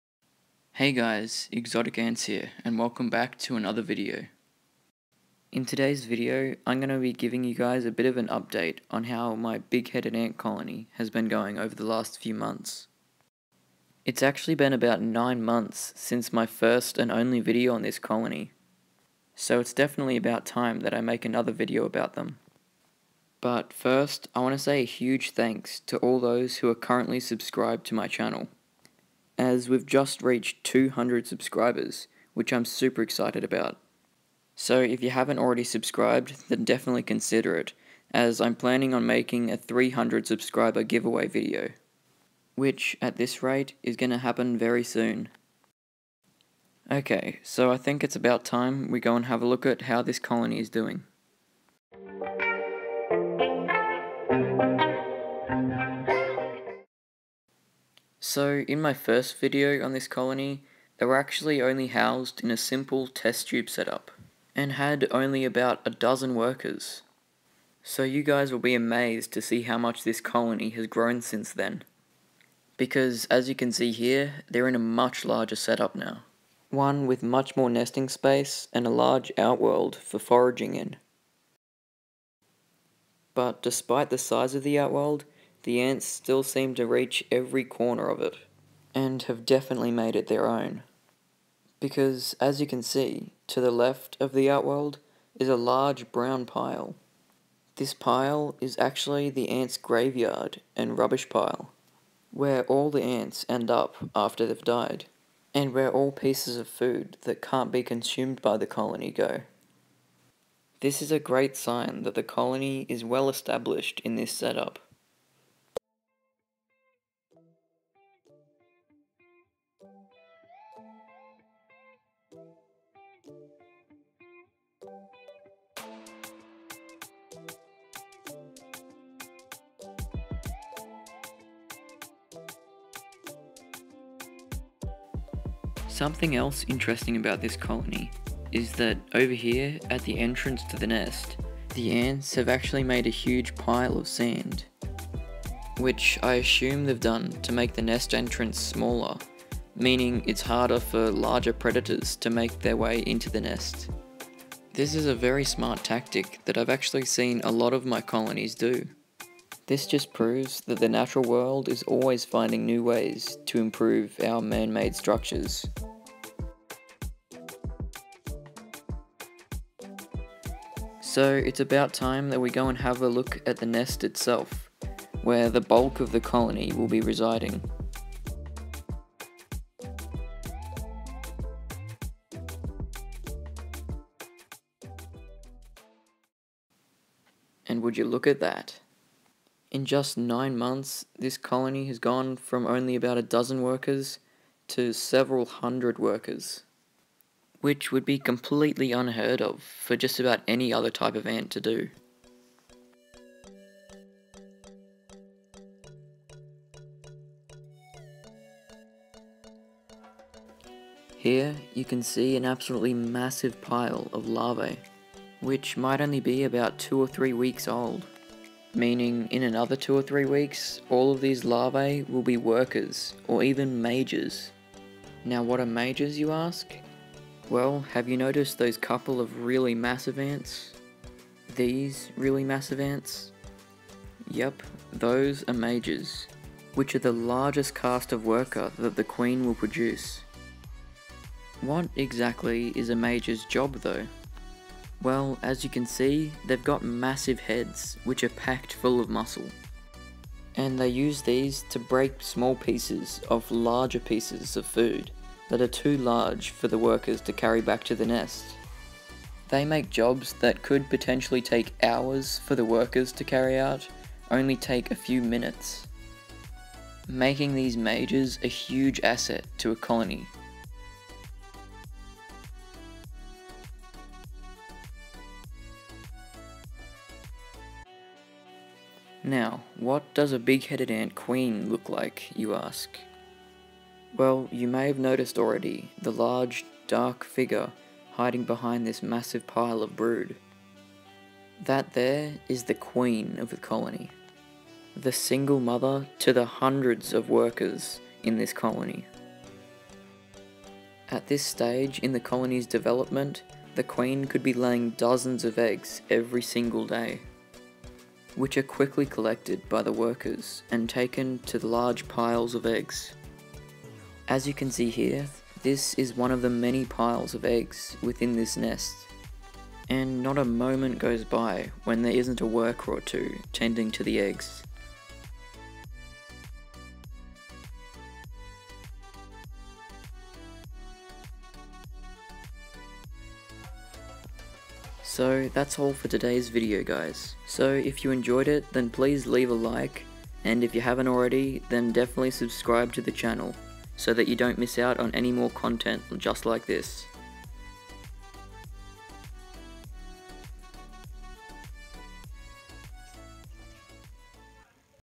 Big Headed Ant Colony, Update #2 (pheidole)
its been 9 months since the last video on this colony, so its about time for an update!